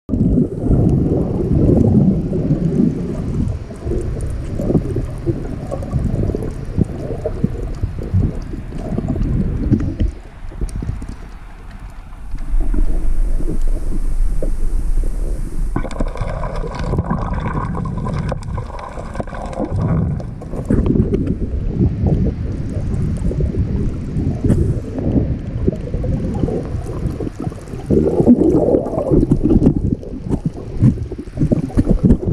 水中ケイブ ＠２ステップ ハワイ島
野生のイルカと3,000回以上泳いできた野崎友璃香の各種無料コンテンツはこちらから→

マーメイドが棲んでいそうな溶岩とコーラルの水中ケイブ（洞窟）